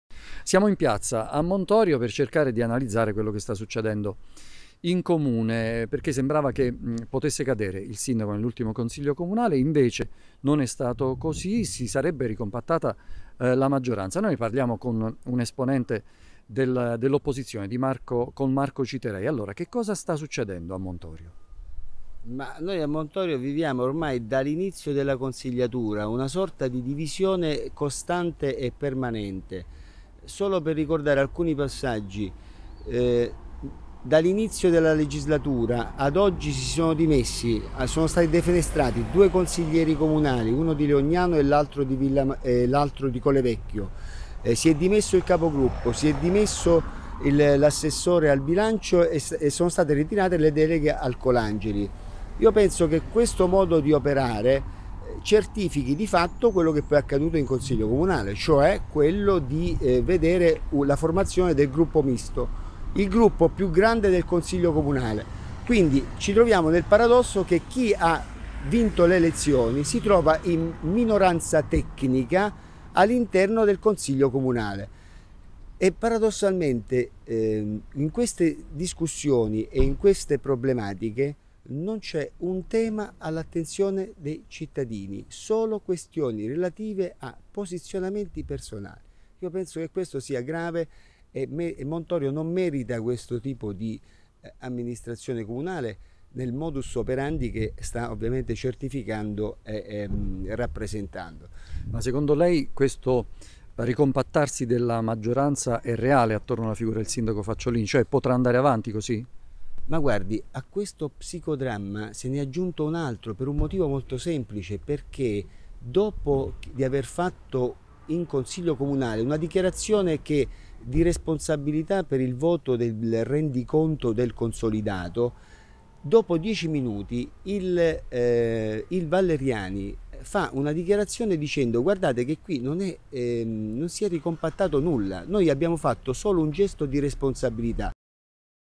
Montorio, il sindaco Facciolini resta in bilico nonostante il voto in Consiglio
| SUPER J |

Contatti Redazione:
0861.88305